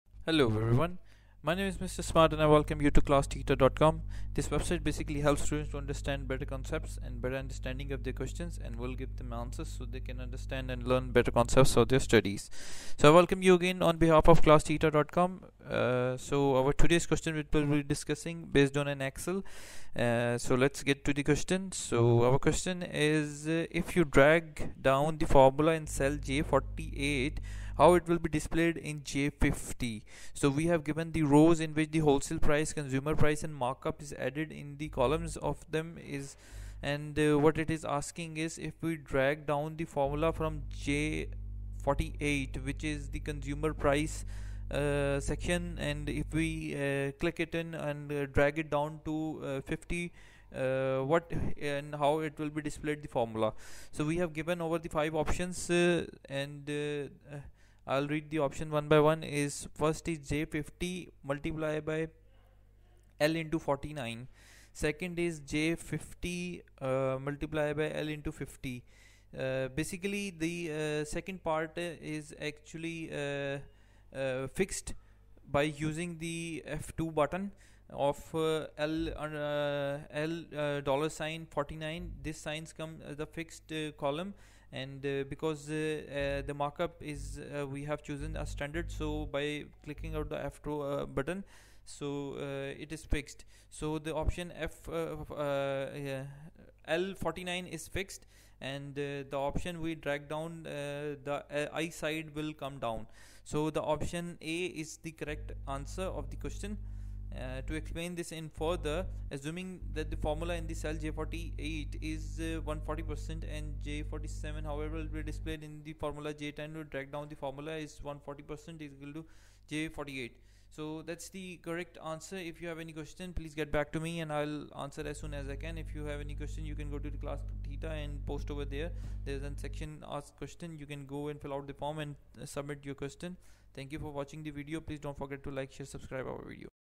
If you drag down the formula in Cell J48, how will it be displayed in Cell J50?
If you drag down the formula in Cell J48, how will it be displayed in Cell J50? Markup 140% 115% 47 Wholesale Price Consumer Price 48 $9.99 148*SLS49 49 $13.99 50 $25.99 SELECT ONLY ONE 150 $L$49 =150"L$50 J50 L$49 J50 L$48 =150*$L$50